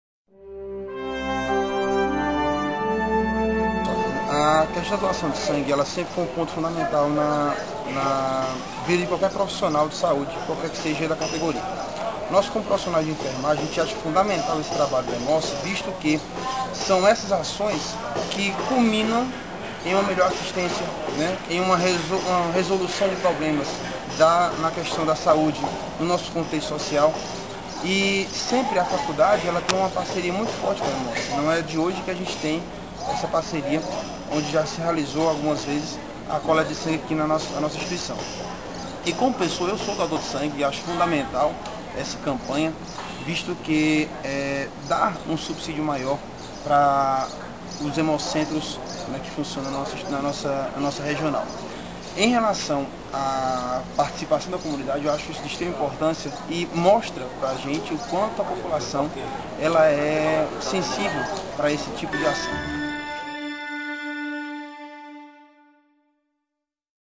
Icó é Notícia: Campanha doação de sangue em Icó - 2011- Parte IV
Entrevista realizada com o coordenador do curso deTécnico em Enfermagem, Hildons Leandro, durante a realização da Campanha de doação desangue realizada nas dependências da Faculdade, no dia 20/01/11.

O evento coletou 86 bolsas de sangue e 32 coletas de medula óssea e teve a realização do Hemoce regional Iguatu, contando com o apoio a Secretaria de Saúde do Icó e parceiros a Associação Filhos e Amigos de Icó (AMICÓ), Leo e Leo Júnior, além da Faculdade Vale do Salgado (FVS) e Macavi.